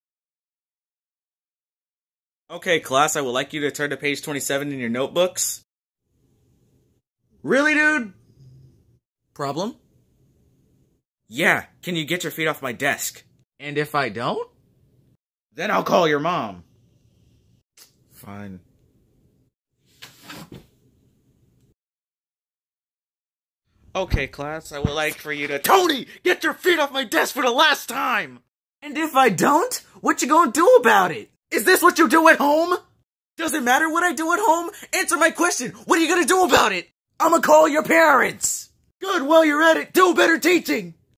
[Fantasy vs. Reality] When the teacher asks "Is this what you do at home?"
Just something I wanted to upload for the week or so

(I dont post much ;-;)